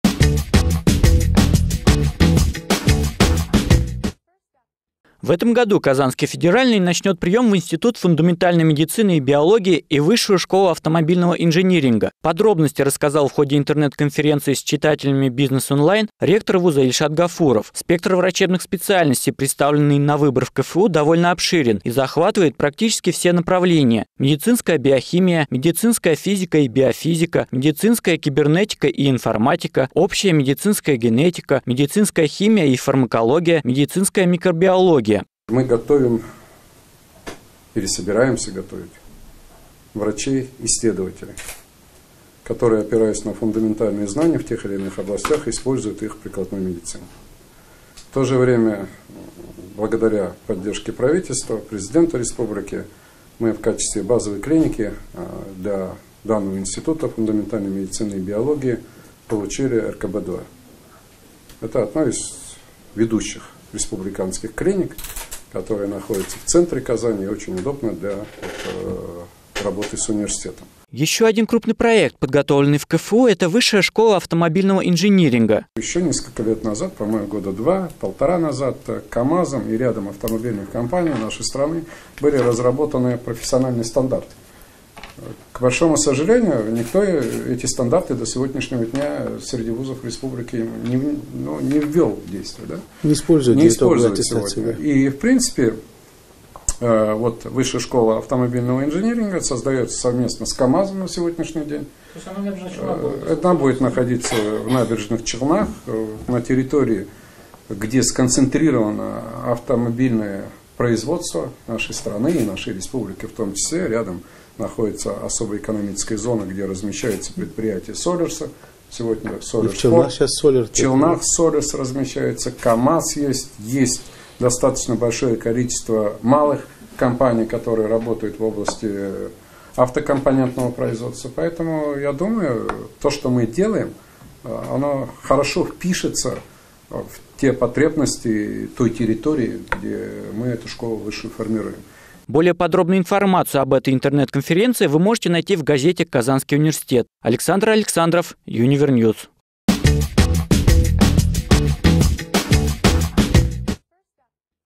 КФУ запускает 2 масштабных проекта.
Интернет-конференция Ильшата Гафурова с читателями "Бизнес-Онлайн"